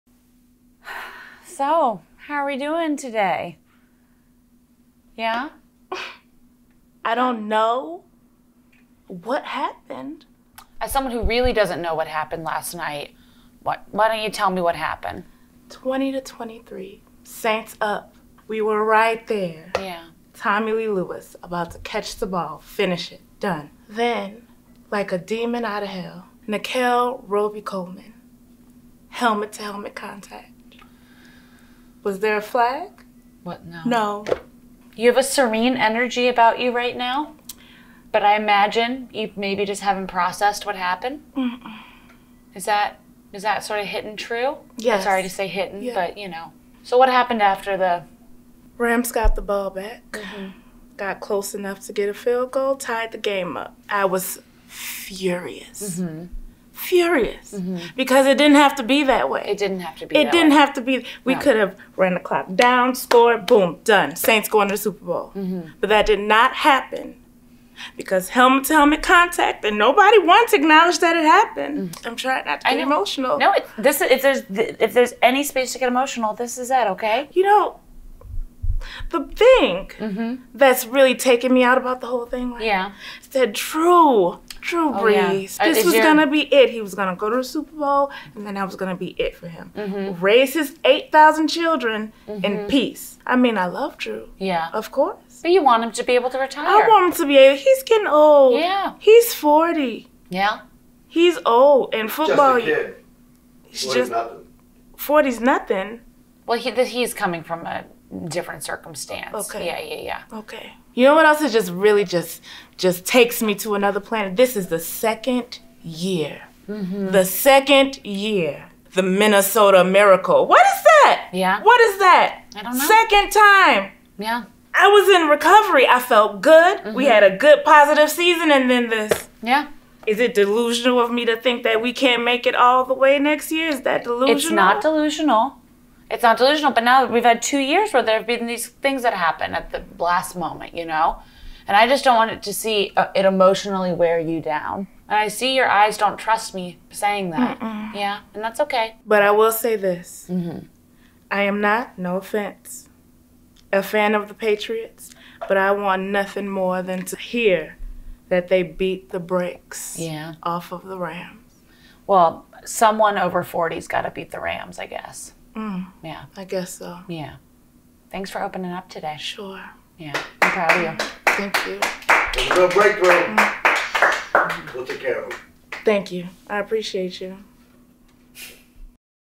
NFL FAN THERAPY: Saints Fans Have No Love For The Refs
After the officials missed a blatant pass interference call near the end of regulation, Saints fans are blaming the refs for their loss to the Rams. After last year's Minnesota Miracle, this is all a bit too much for the New Orleans fans to bear.

CAST
Therapist: Kady Ruth Ashcraft
Pats Fan: Randy Gardner
Bills Fan: Jon Hudson
Chiefs Fan: Gabe McKinley
Redskins Fan: Courtney Maginnis
Eagles Fan: Jim Dodge
Saints Fan: Roxie E. Wilson
Colts Fan: Geoffrey Stevens
Rams Fan: Aaron Weininger

CREW
Cinematographer/Editor: Geoffrey Stevens
2nd Camera & AD: Michelle Byron
Sound: Michael Bain